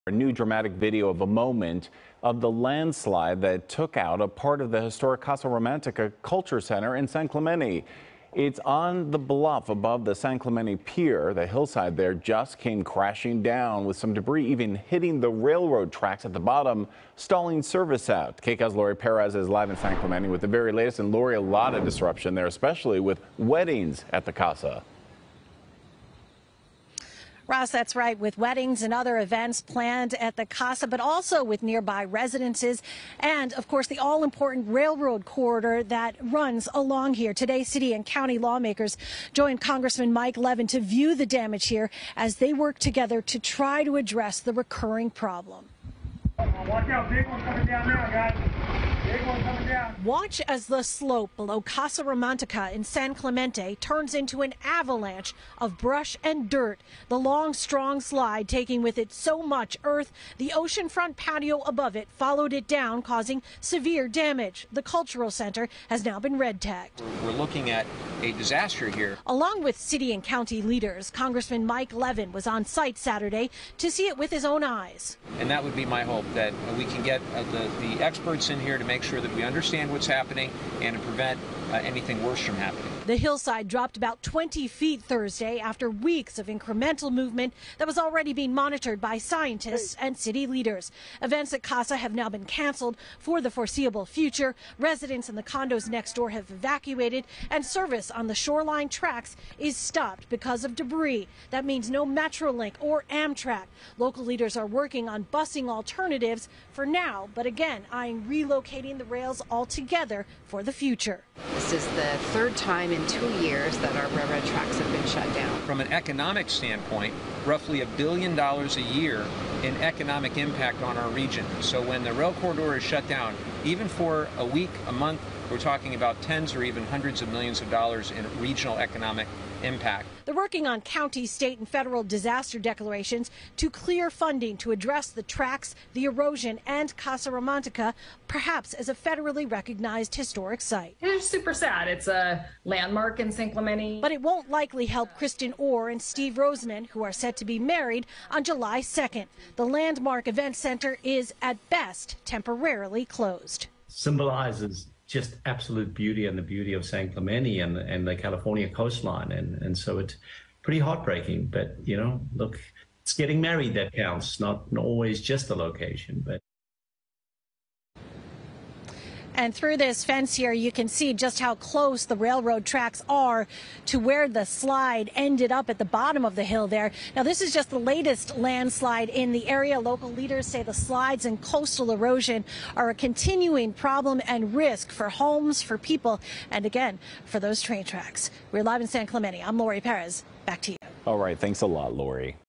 Video shows land continuing to give way underneath Casa Romantica in San Clemente
Laurie Perez reports from San Clemente, where rail operations and community landmark Casa Romantica remain closed as a landslide continues to move days after 20-feet gave way.